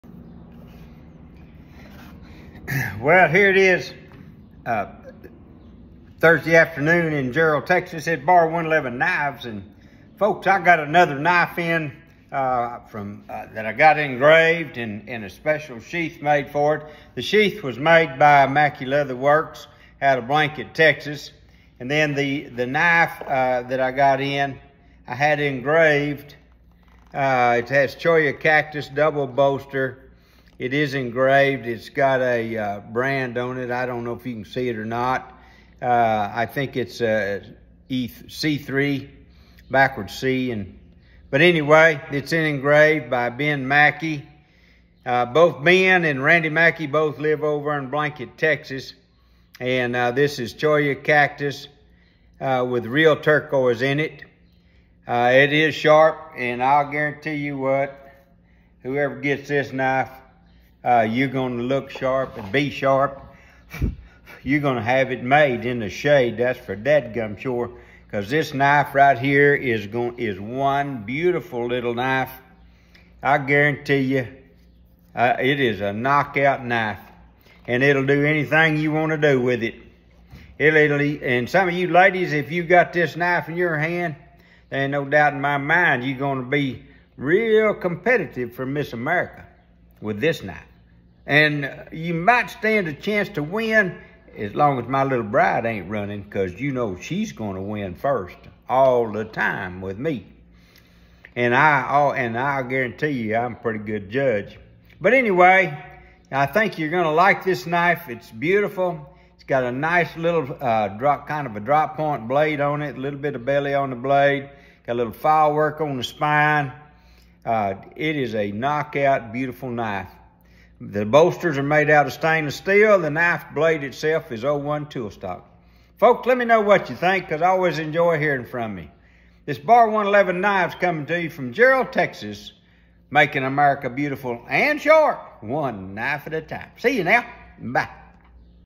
Baroneelevenknives
Cholla cactus with turquoise double bolsters engraved with custom sheat.
You are not only going to look sharp you will be sharp